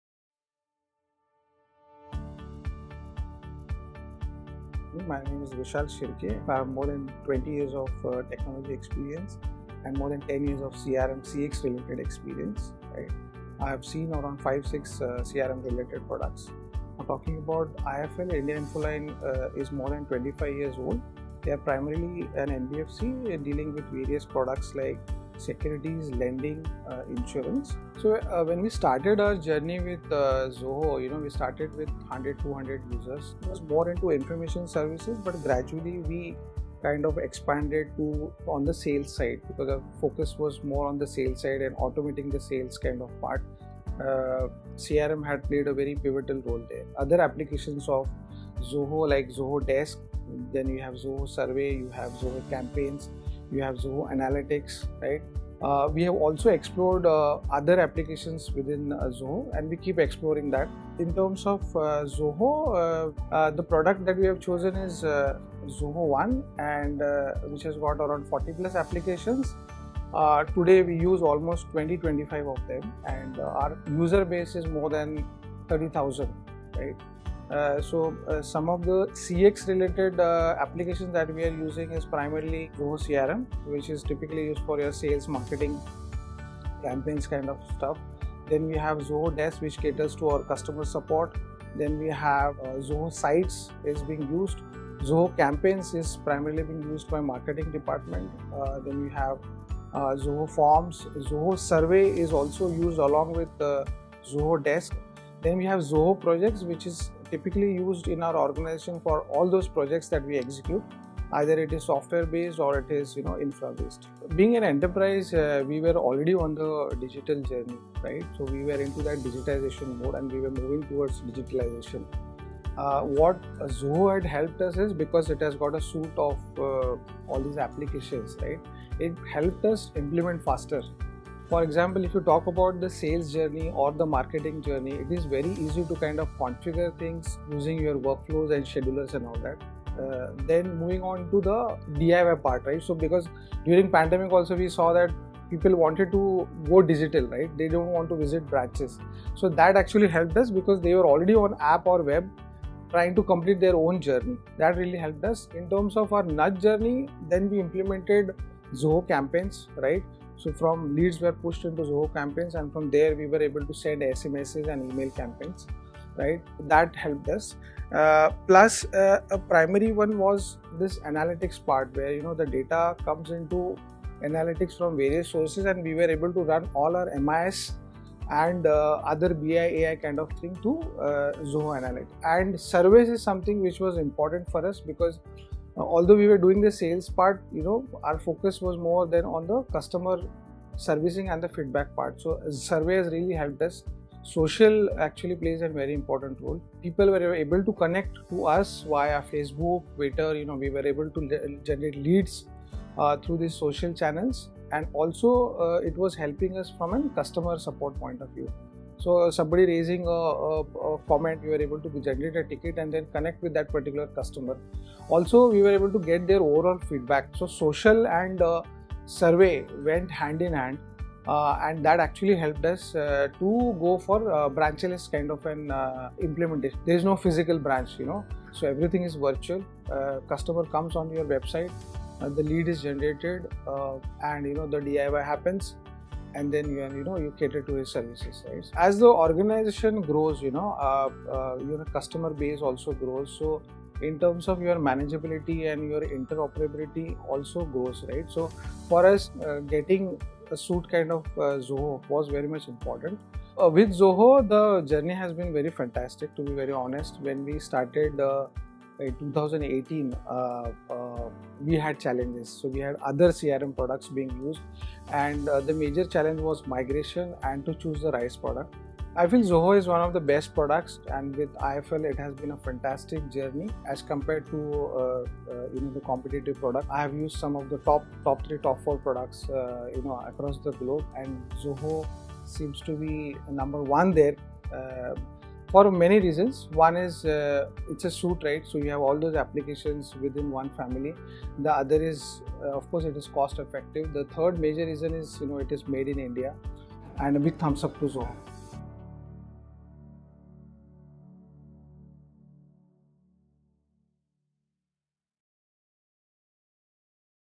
Zoho CRM helps IIFL, a financial company to automate their sales and marketing
IIFL, an Indian based financial company automates its sales and marketing processes using Zoho CRM.

At Zoholics Mumbai, 2022, Mr. Vishal Shirke, Vice President and CRM Product Head at IIFL, talks about how Zoho CRM has helped them digitize their business to keep up with their customers' expectations.